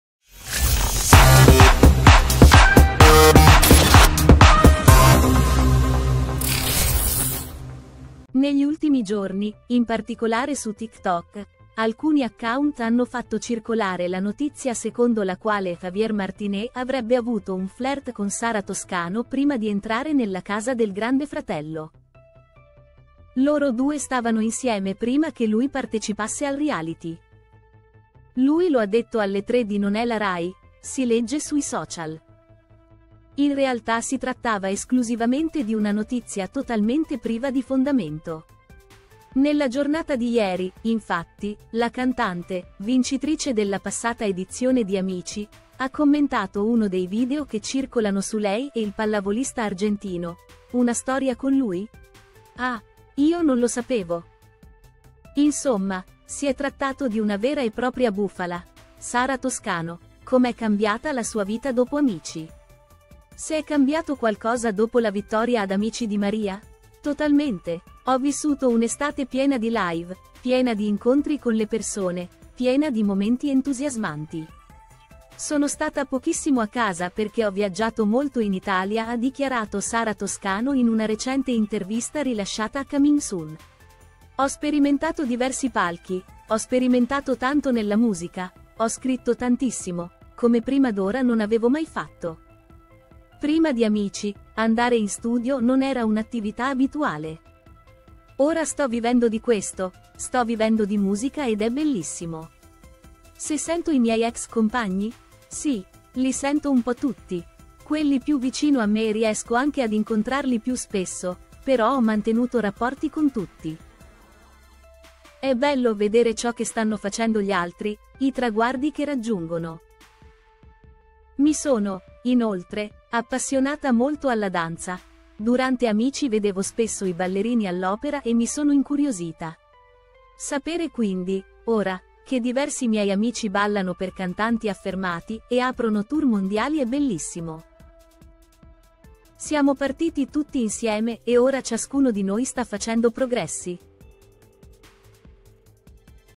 Grande Fratello, Javier Martinez ha avuto un flirt con Sarah Toscano La cantante rompe il silenzio
Negli ultimi giorni, in particolare su TikTok, alcuni account hanno fatto circolare la notizia secondo la quale Javier Martinez avrebbe avuto un flirt con Sarah Toscano prima di entrare nella Casa del Grande Fratello.

“Loro due stavano insieme prima che lui partecipasse al reality. Lui lo ha detto alle tre di Non è la Rai”, si legge sui social. In realtà si trattava esclusivamente di una notizia totalmente priva di fondamento. Nella giornata di ieri, infatti, la cantante, vincitrice della passata edizione di Amici, ha commentato uno dei video che circolano su lei e il pallavolista argentino: “Una storia con lui? Ah, io non lo sapevo”. Insomma, si è trattato di una vera e propria bufala.